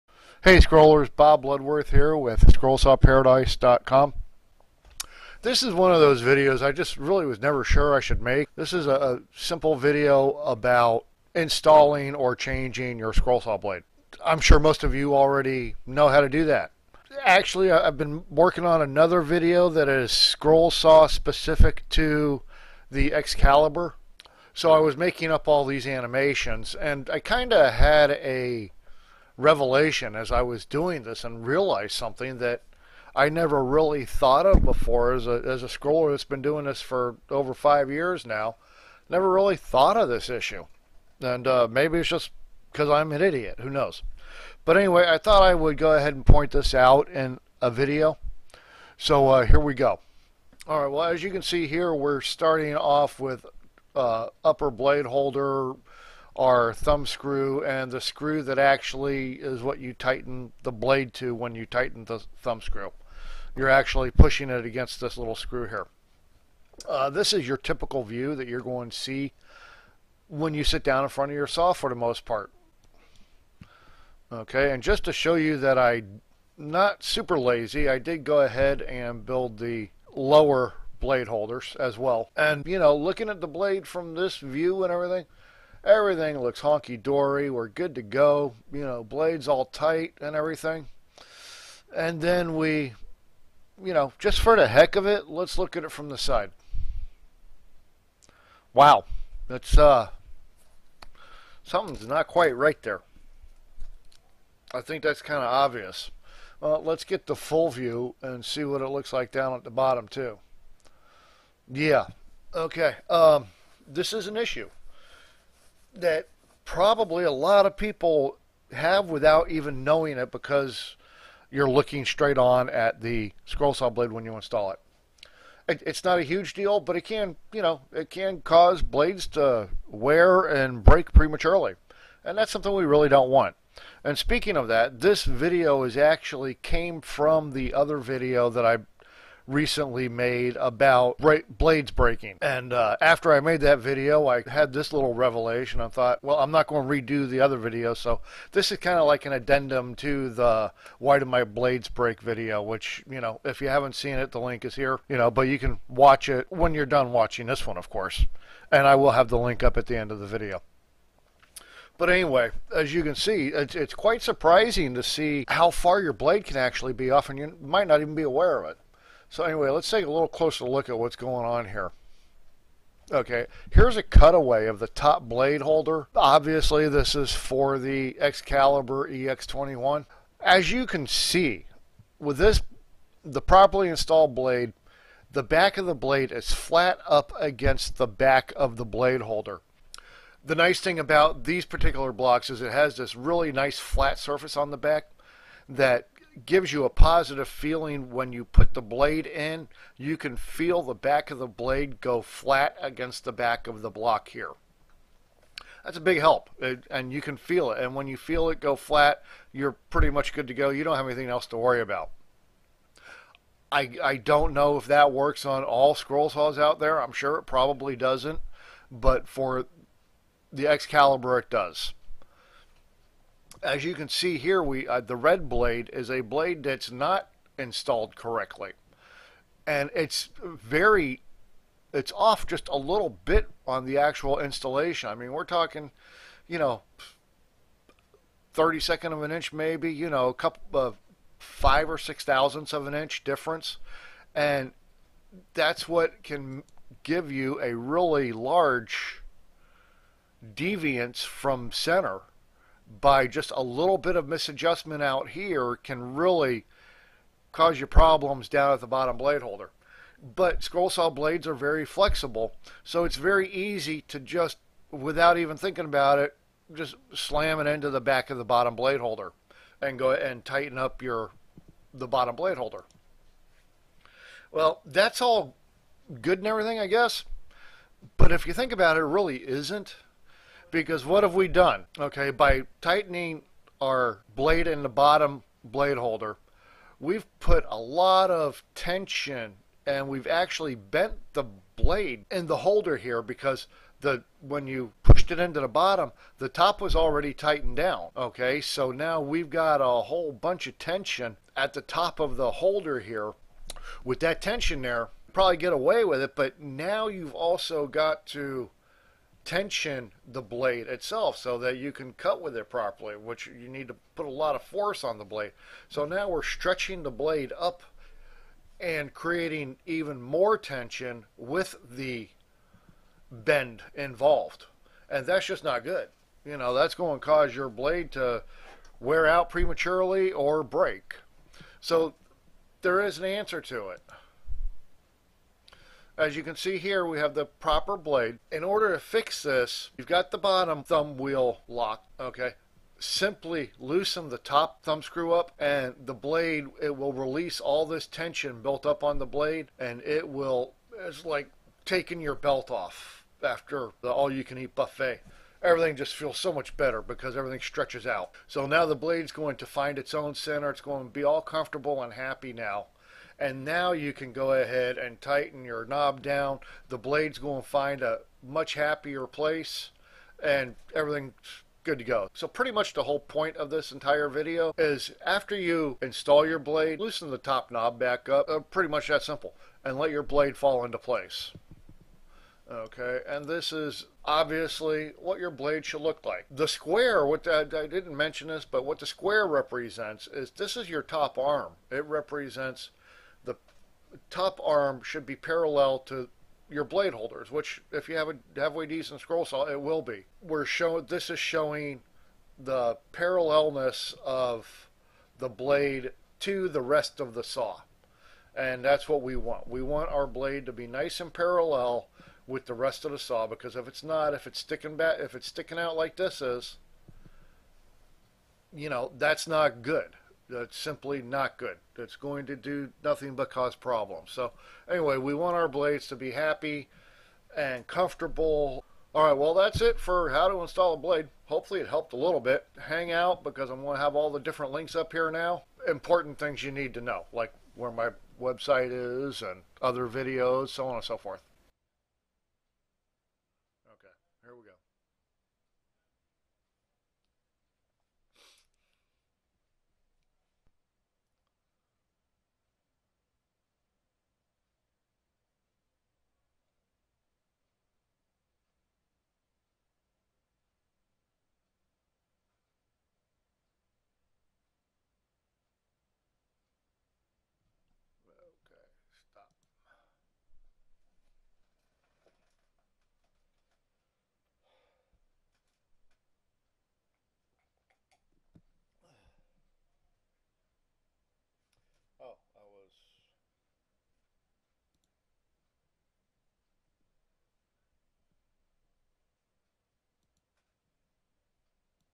Scroll Saw Blade Installation Animation
A short video on some possibly over looked details to installing scroll saw blades.
Animation made with Google Sketchup